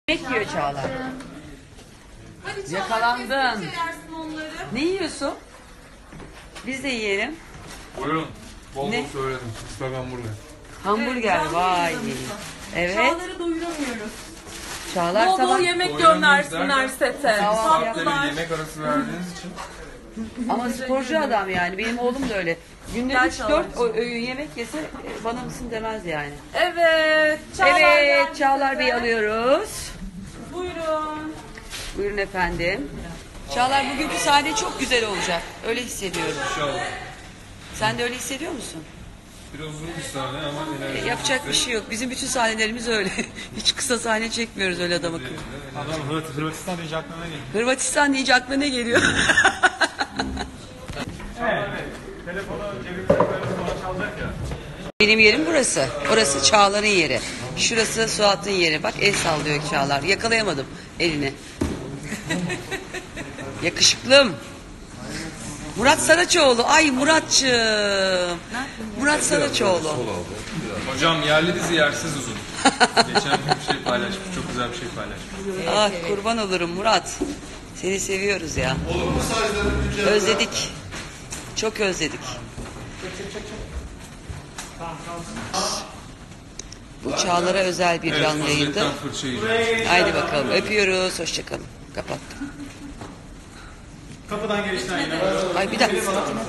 Çaglar Ertuğrul Canli yayin live broadcast Fazilet Hanim ve Kizlari
Live broadcast in Fazilet Hanim ve Kizlari By Nazan Kesal English translation by Otekiprofilim thanks a lot 🙏🏼